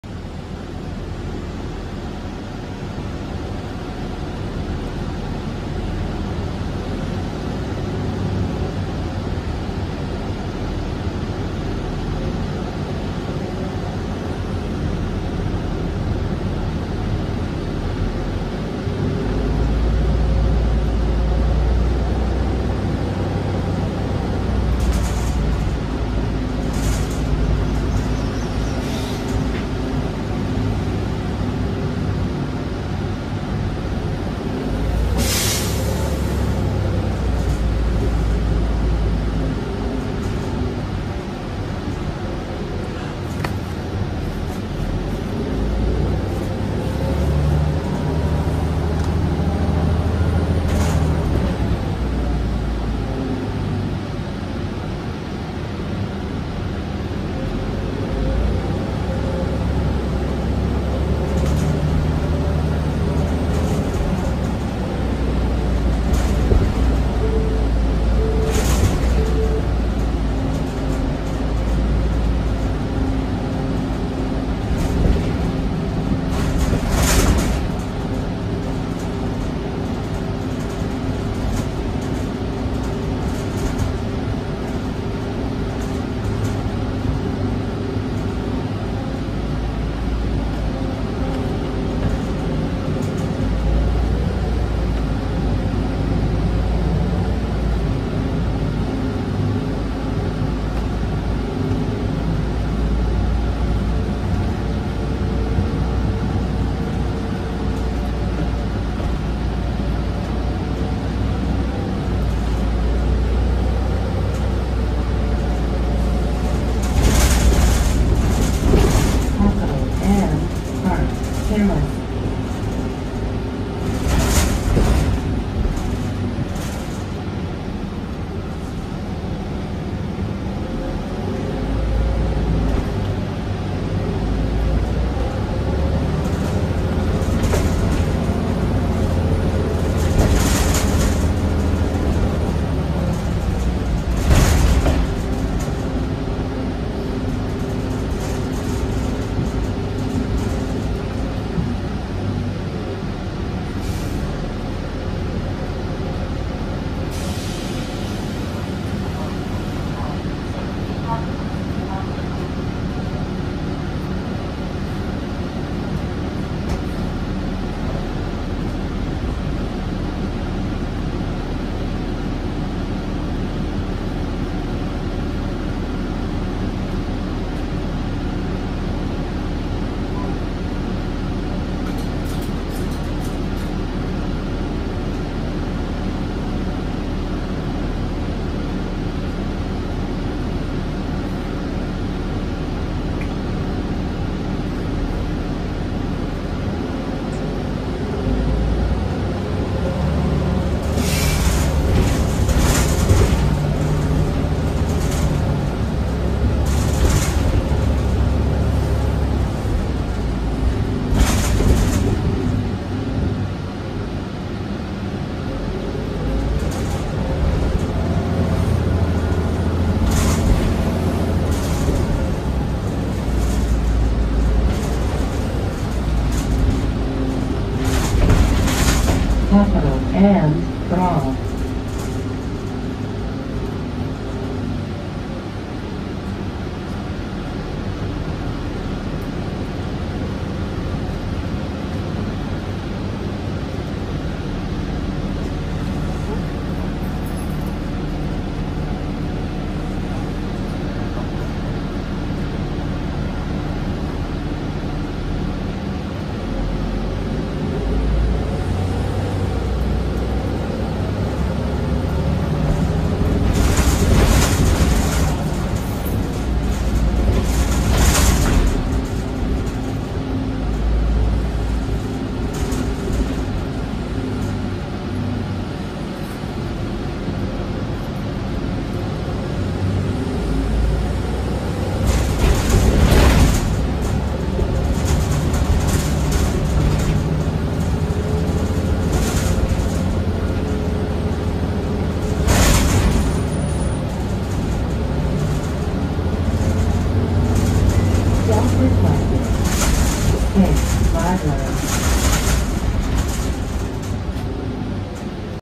Ride on 2014 Gillig BRT Hybrid 1431 on route 161 capitol Avenue ￼
Cummins ISB6.7
Allison H 40 EP